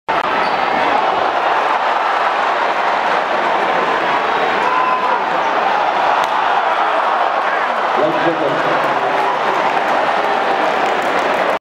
Chris Paul Alley-Oop to Blake Griffin.wmv
Chris Paul tosses this pretty alley-oop to Blake Griffin against the Hornets.